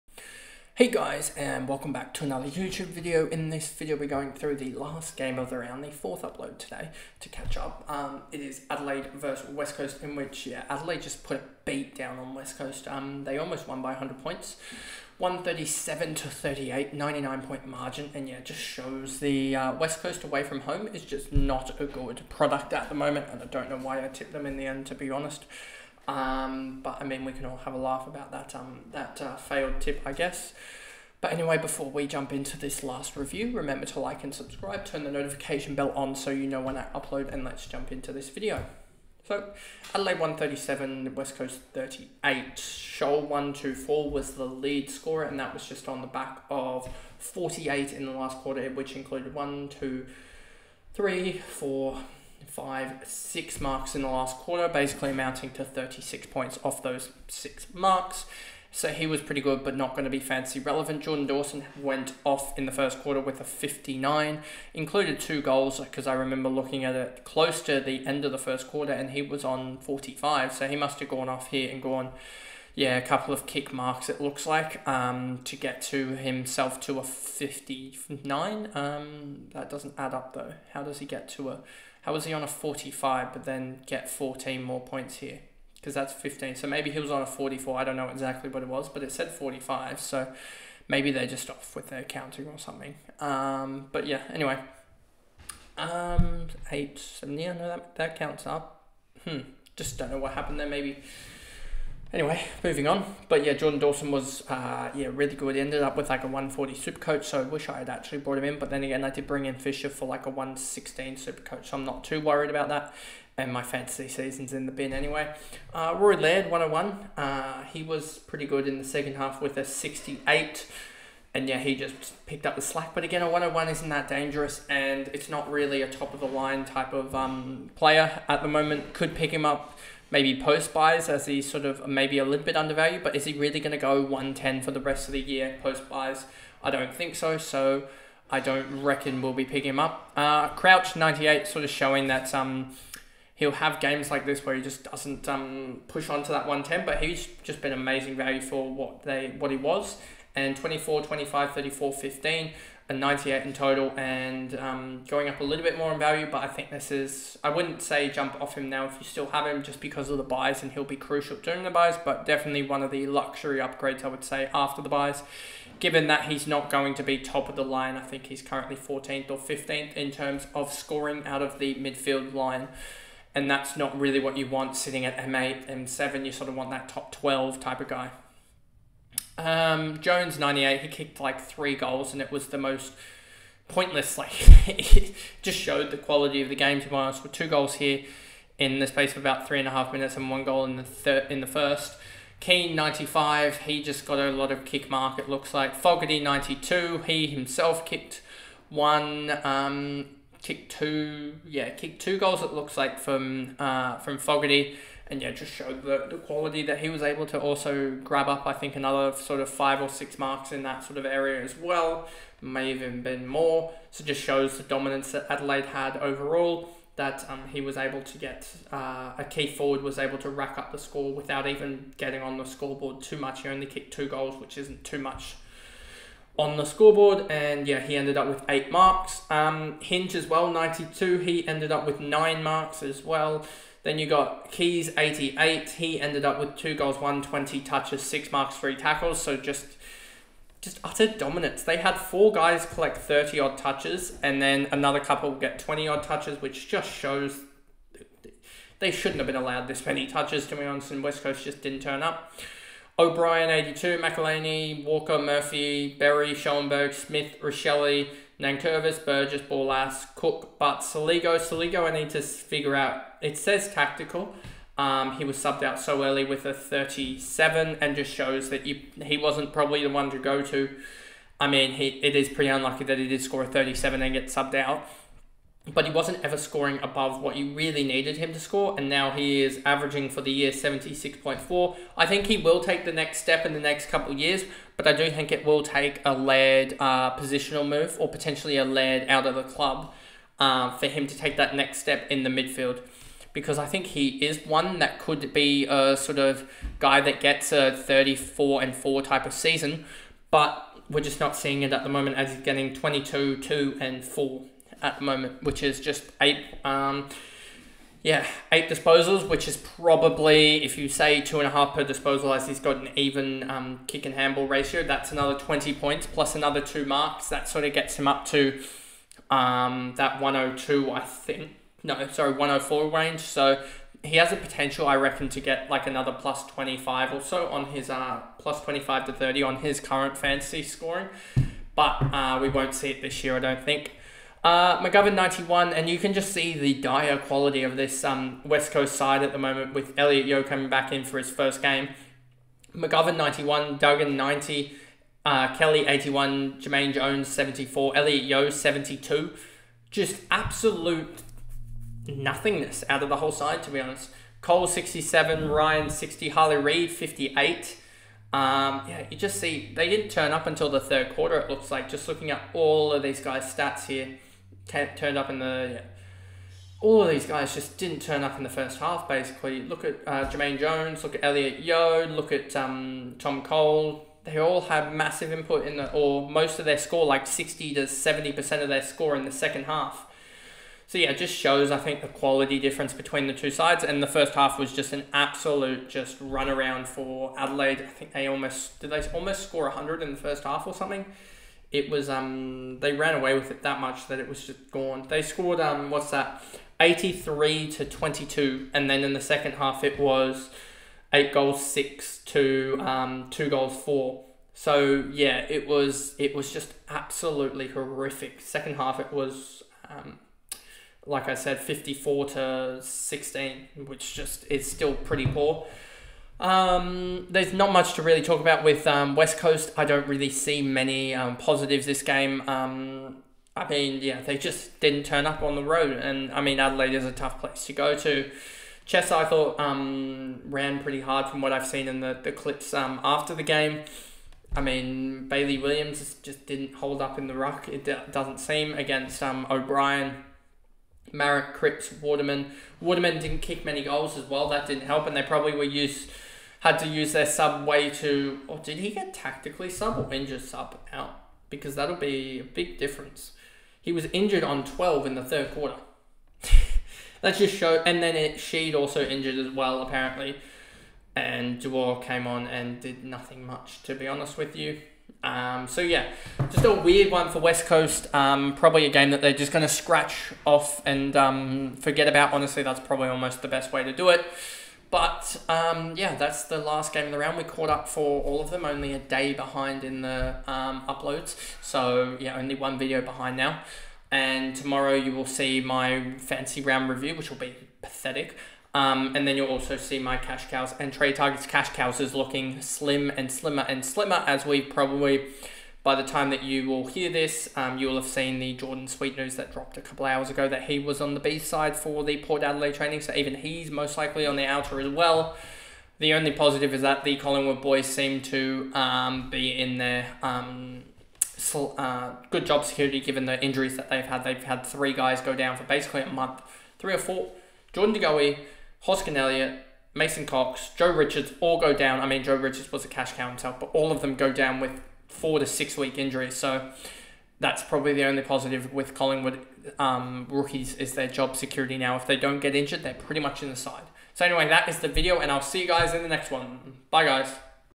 Game Breakdown: Adelaide Crows vs West Coast Eagles AFL Fantasy 2024
Get a detailed breakdown of the Adelaide Crows vs West Coast Eagles AFL game in this fantasy analysis video. Learn about the key players, highlights, and strategies used by the West Coast Eagles in this one sided match!